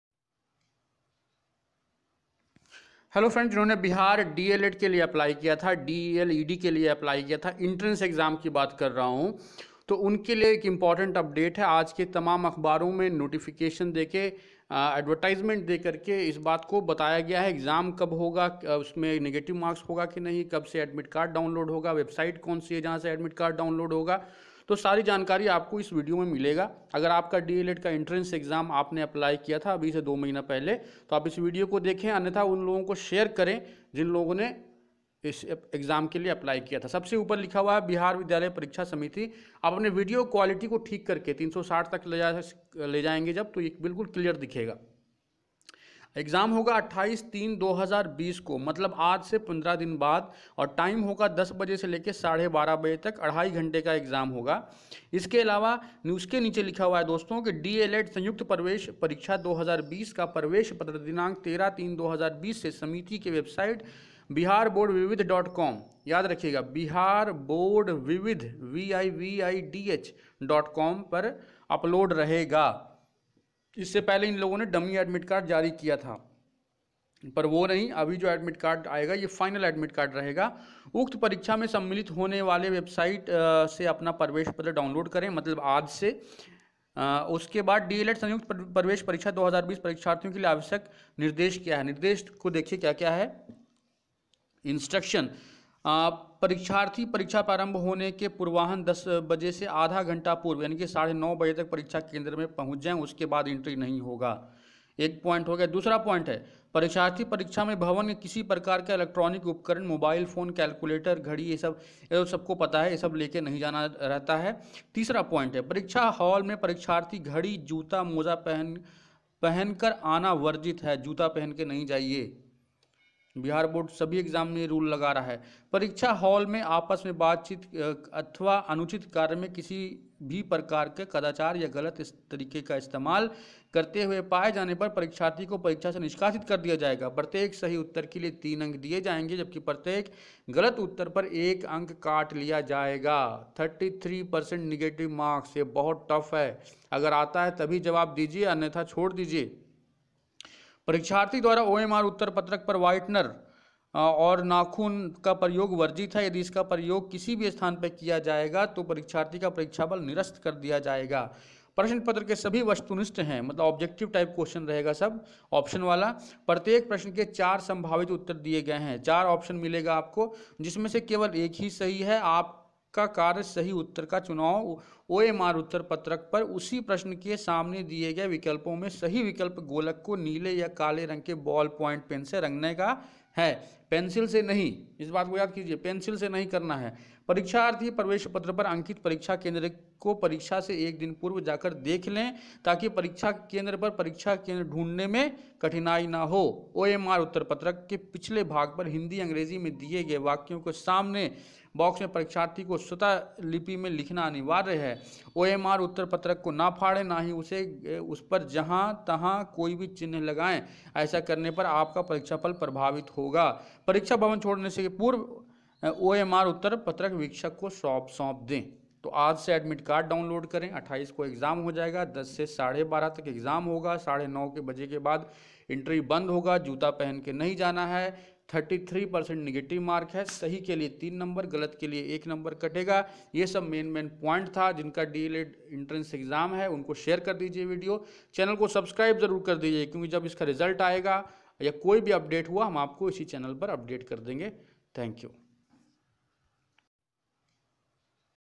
D.el.ed Entrance Exam 2020, Bihar Board Deled Exam Date, Admit Card, Question Paper, Cut Off 2020
This video speaks about Bihar Board D.el.ed exam date and admit card. Check video for more information about this Bihar board exam.